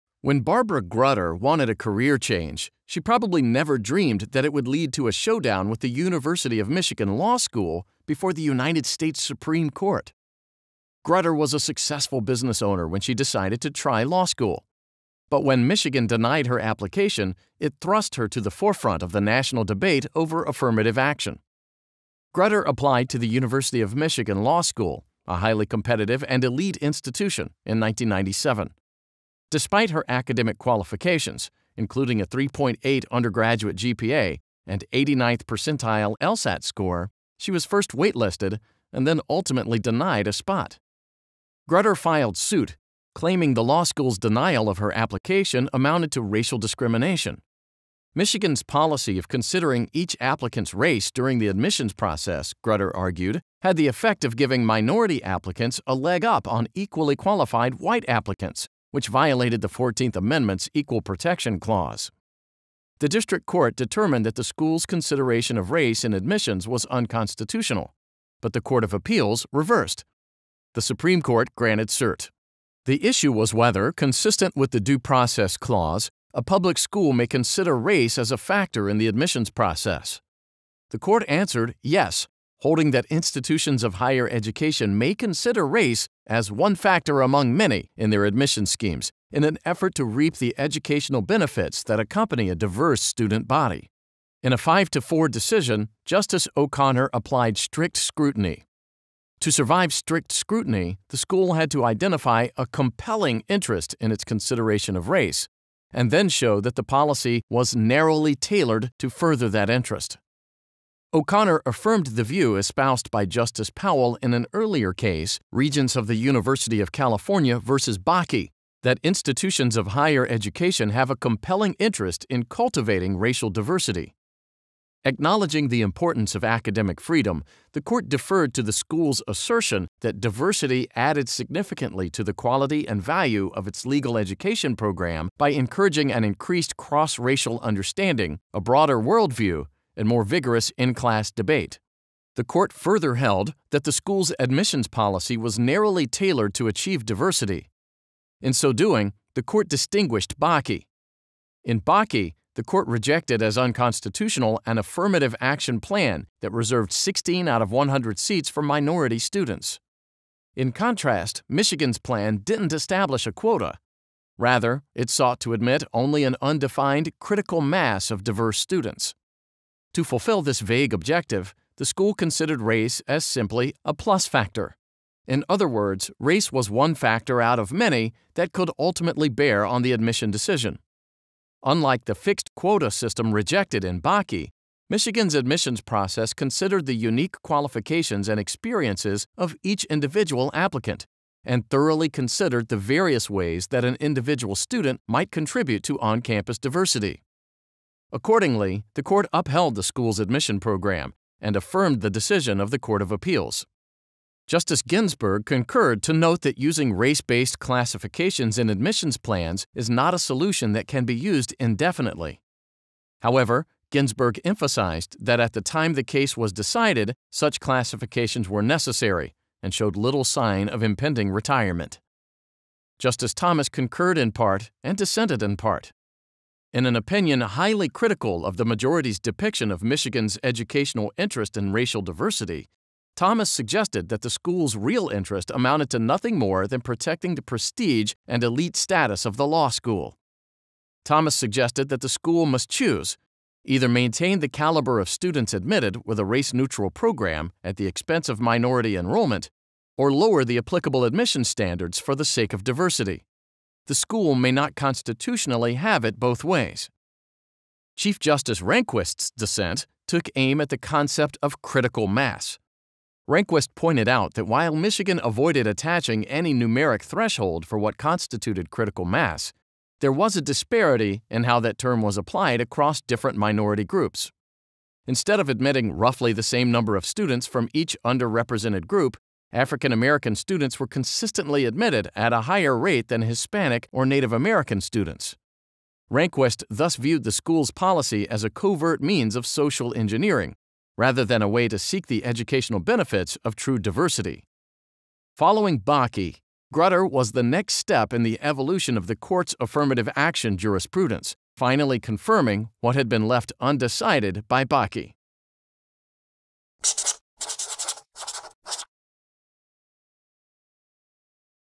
Grutter v. Bollinger Case Brief Summary | Law Case Explained
Grutter v. Bollinger | 539 U.S. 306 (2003)
When Barbara Grutter wanted a career change, she probably never dreamed that it would lead to a showdown with the University of Michigan Law School before the United States Supreme Court. Grutter was a successful business owner when the denial of her law school application thrust her to the forefront of the national debate over affirmative action.

Grutter applied to the University of Michigan Law School, a highly competitive and elite institution, in 1997. Despite her academic qualifications, including a 3.8 undergraduate GPA and 89th percentile LSAT score, she was first waitlisted, and then ultimately denied a spot.

Want more details on this case? Get the rule of law, issues, holding and reasonings, and more case facts

Have Questions about this Case?